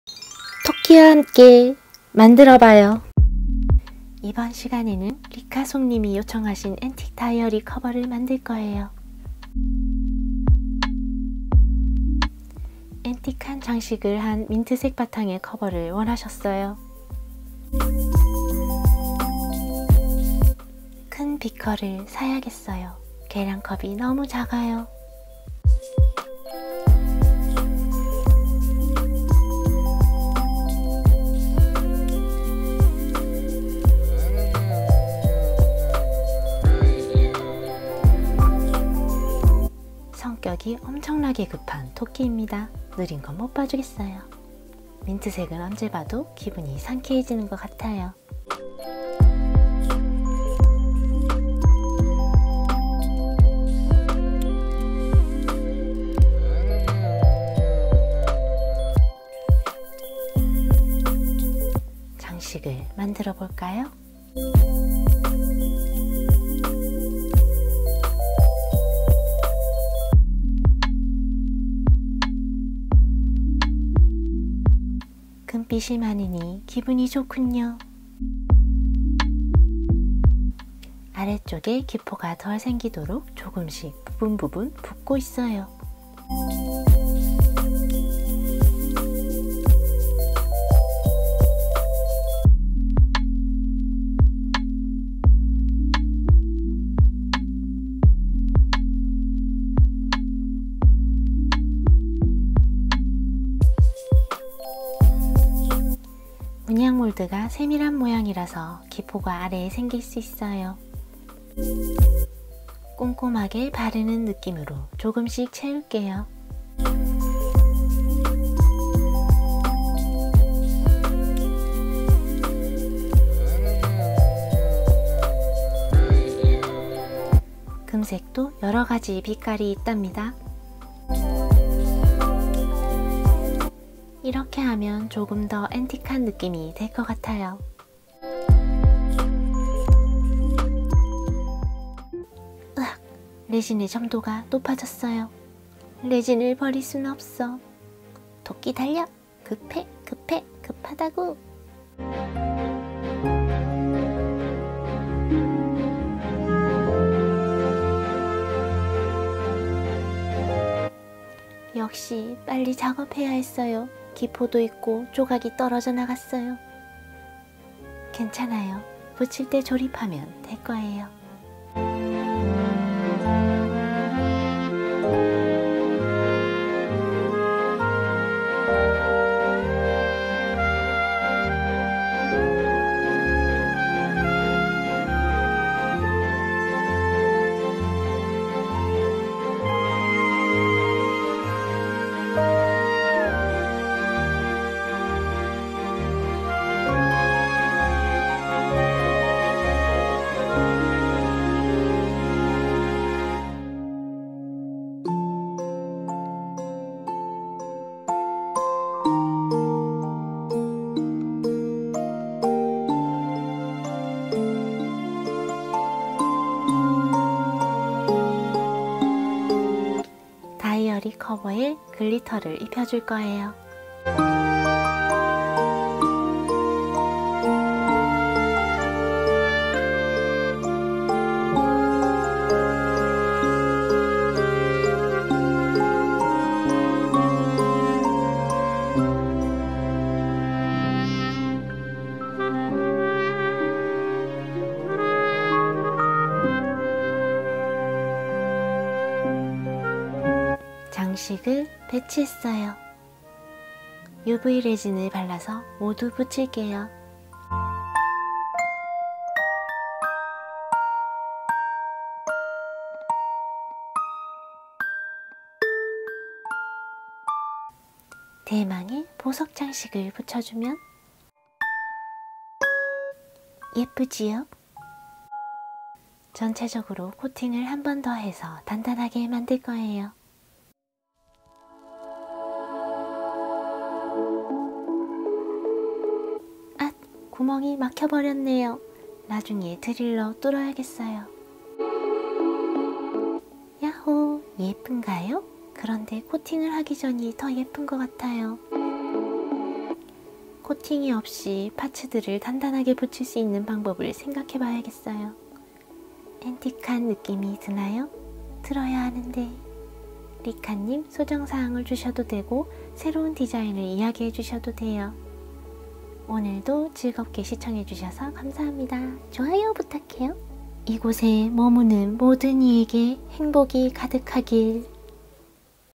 리미티드 다이어리 커버 만들기
오늘은 구독자(Rika SONG)님이 요청하신 엔틱한 느낌의 다이어리 커버를 만들었어요.

인스타에 놀러오시면
토끼가 유튜브에서 우주로 날려버린 다른 작품들도
감상하실 수 있어요!

감사합니다 ٩(ˊᗜˋ*)و 💜